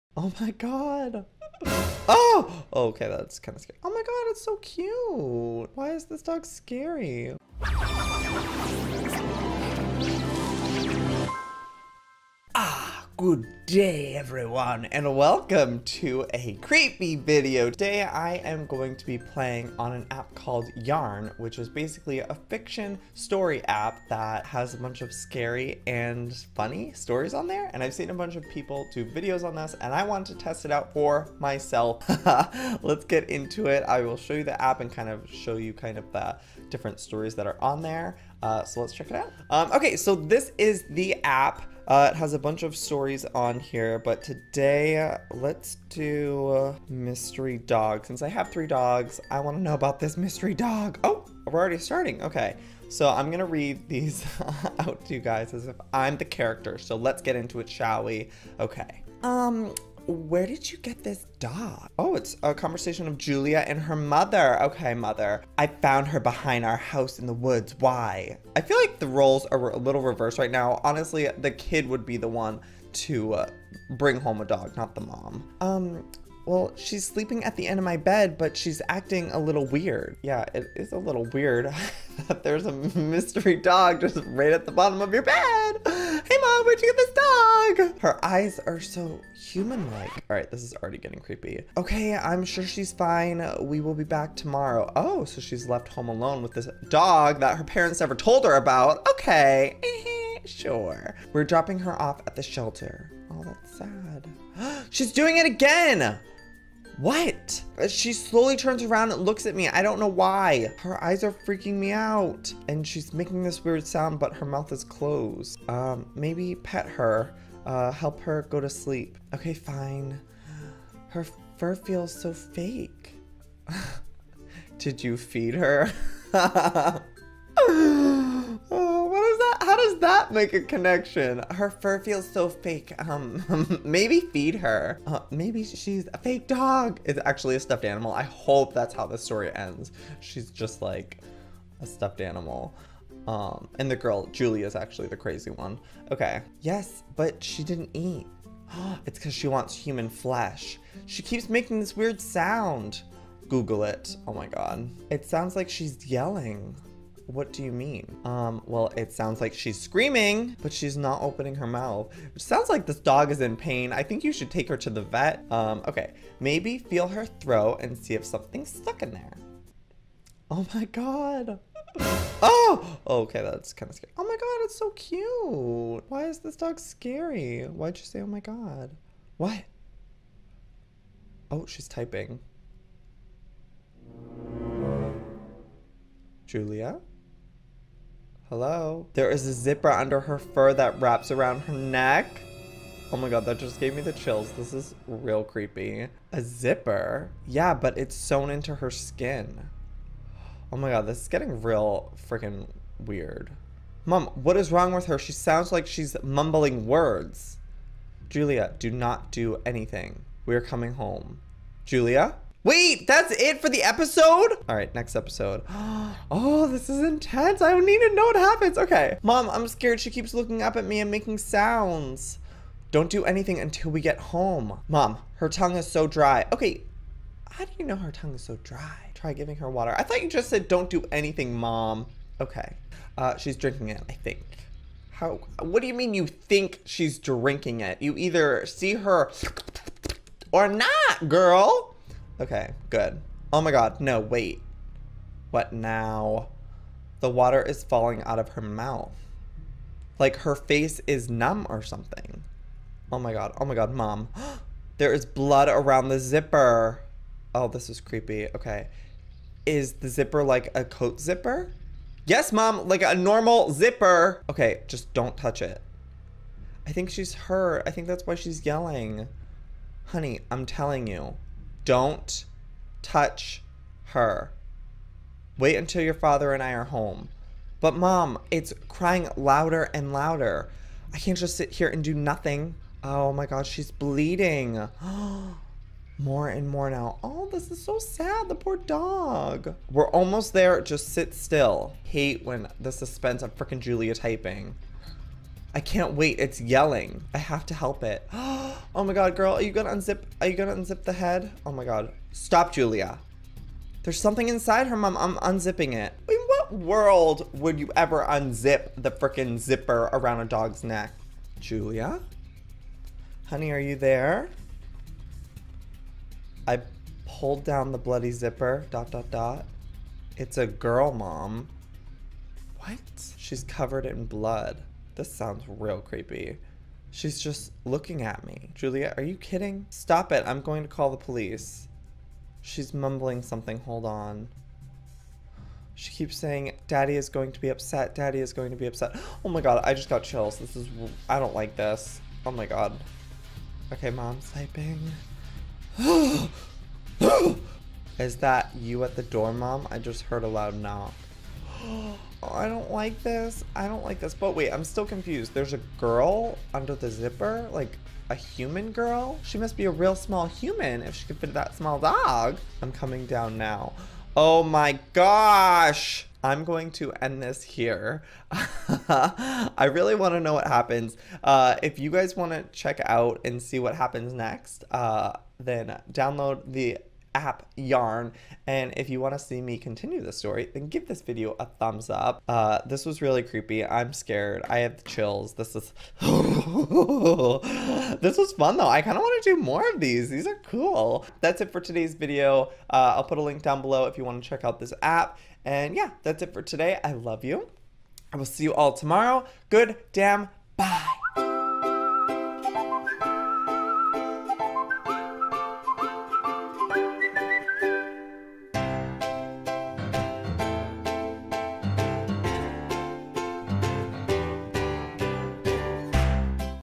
CREEPY TEXT CONVERSATIONS! (Yarn)
I READ SOME SUPER CREEPY TEXT CONVERSATIONS!
Download the Yarn: Chat Fiction app to read the creepiest chat stories with dedicated